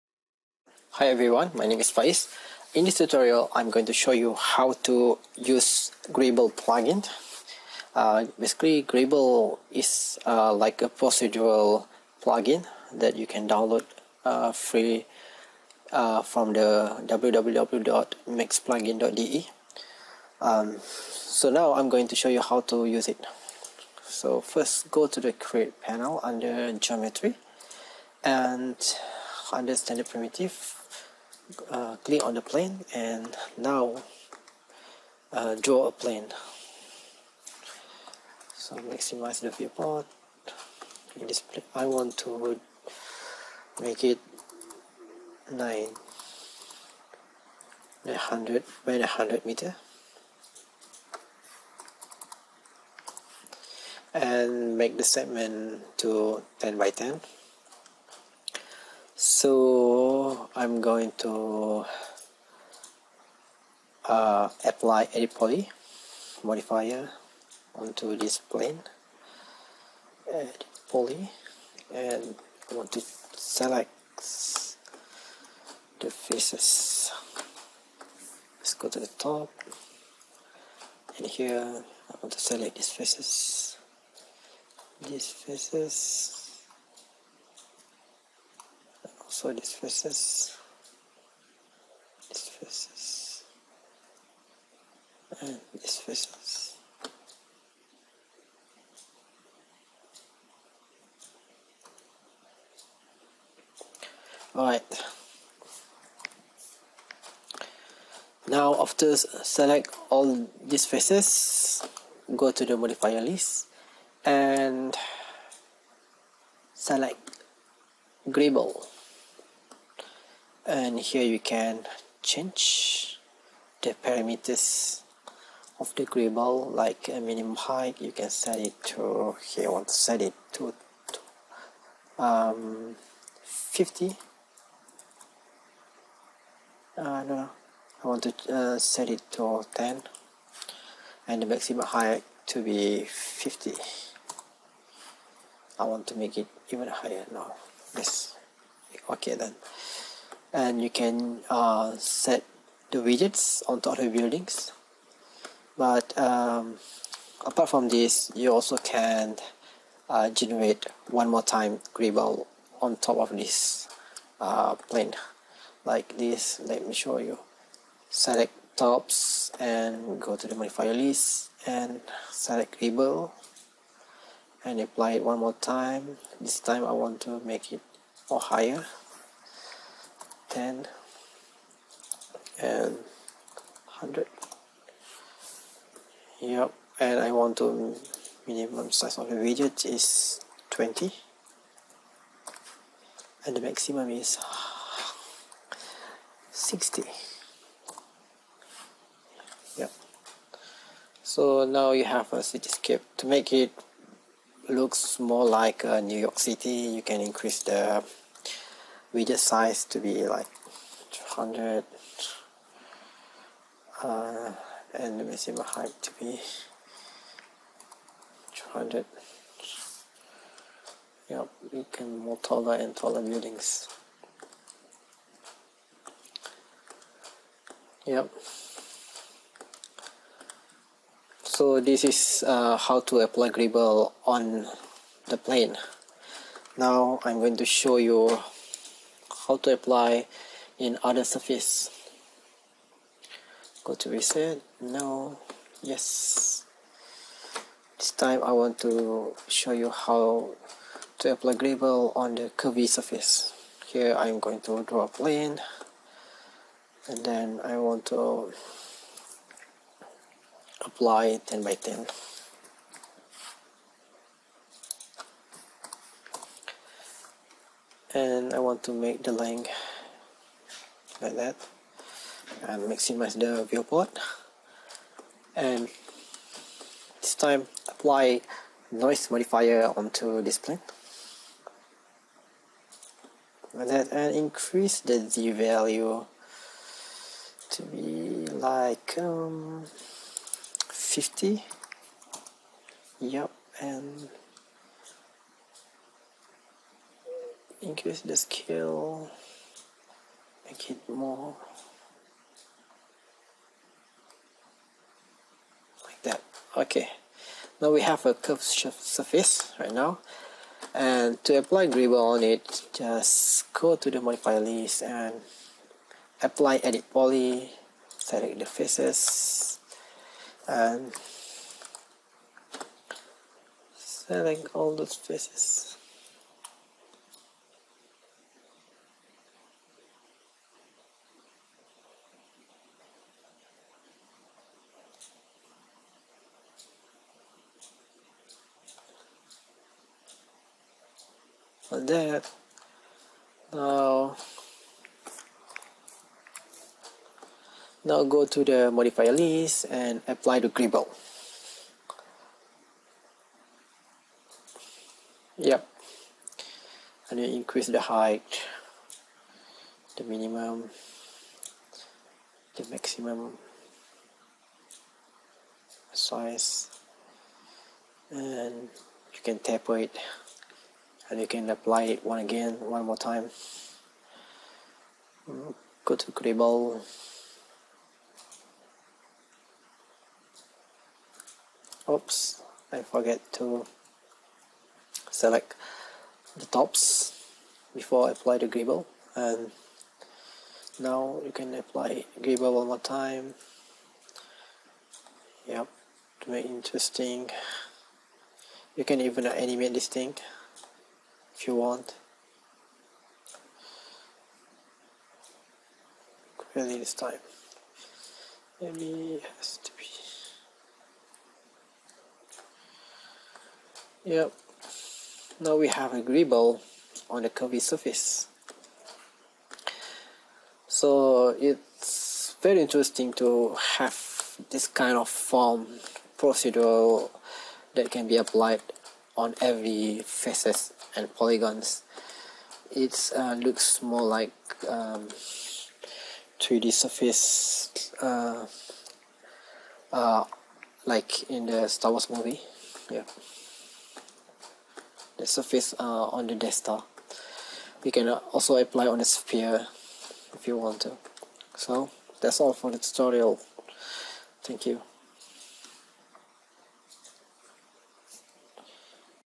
02 How to use Greeble
In this tutorial i'll show you how to create instant city blocks by using Greeble